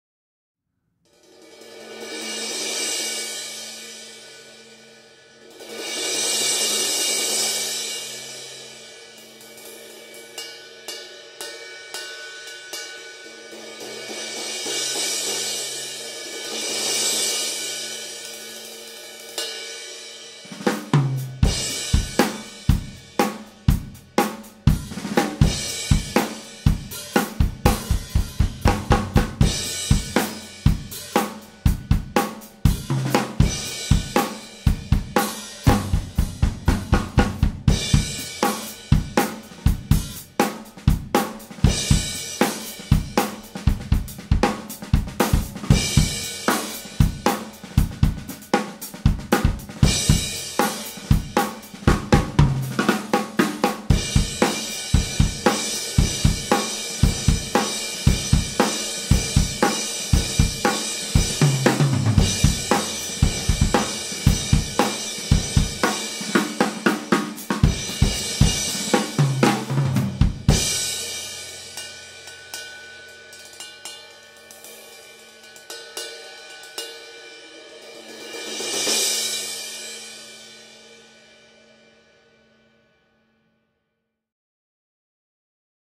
Zildjian 18" A Custom Projection Crash Cymbal - 1415g (A20584-1062219E)
Zildjian 18" A Custom Projection Crash Cymbal at Memphis Drum Shop.

Other gear used in this video:
• Cymbals (From drummer's L to R): Zildjian 14" K Hi-Hats, Zildjian 20" A Custom Ride Cymbal